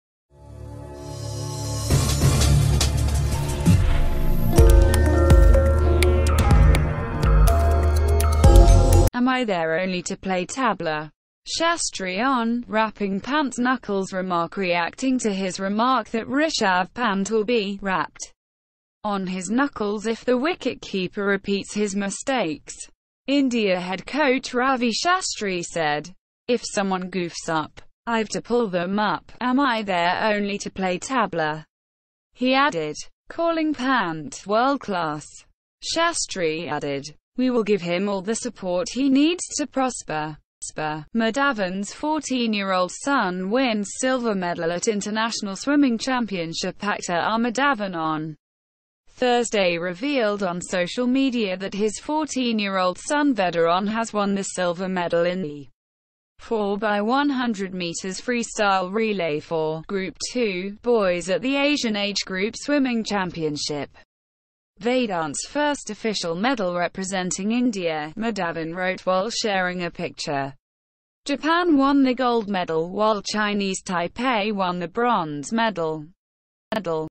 unknown
--- Am I there only to play tabla: Shastri on 'rapping Pant's knuckles' remark ---
  Reacting to his remark that Rishabh Pant will be 'rapped on his knuckles' if the wicketkeeper repeats his mistakes, India head coach Ravi Shastri said, "If someone goofs up, I've to pull them up." "Am I there only to play tabla?" he added. Calling Pant "world class", Shastri added, "We will give him all the support he needs to prosper."
 --- Madhavan's 14-yr-old son wins silver medal at int'l swimming championship ---
  Actor R Madhavan on Thursday revealed on social media that his 14-year-old son Vedaant has won the silver medal in the 4x100m freestyle relay for 'Group II' boys at the Asian Age Group Swimming Championship. "Vedaant's first official medal representing India," Madhavan wrote while sharing a picture. Japan won the gold medal while Chinese Taipei won the bronze medal.